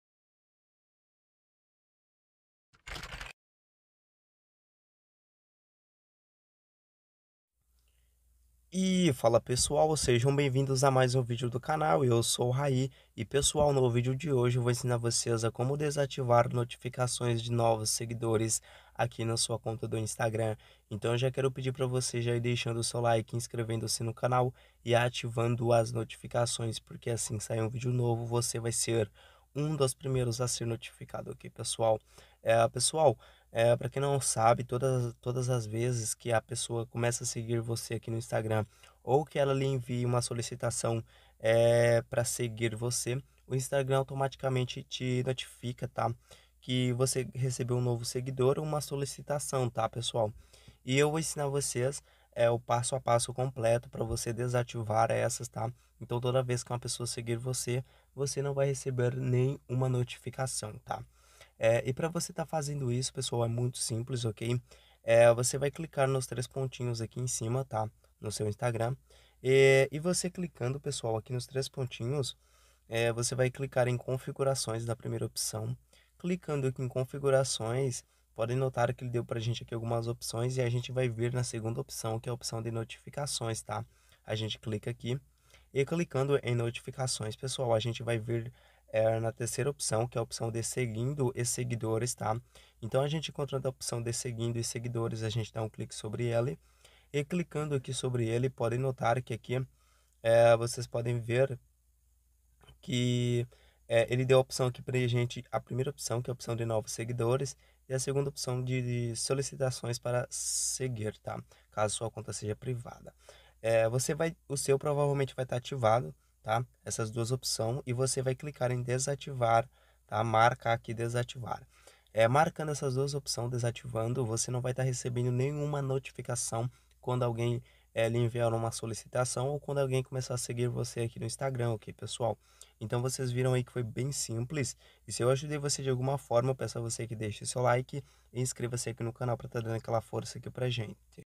Como desativar notificação de seguidores no Instagram
Como desativar notificação de seguidores no Instagram? No vídeo de hoje eu vou mostrar para vocês o passo a passo de como você pode desativar as notificações de seguidores do Instagram.

➥ Tema do vídeo:
"Como parar de receber notificação de seguidores no Instagram"
O Instagram tem a função de notificar quando agente ganhar novos seguidores no aplicativo, mais você sabia que você pode desativar essas notificações do Instagram.

➥ Pesquisa relacionadas
▶ Como desativar notificações do Instagram
▶ Desativar notificações do Instagram

Gratidão:)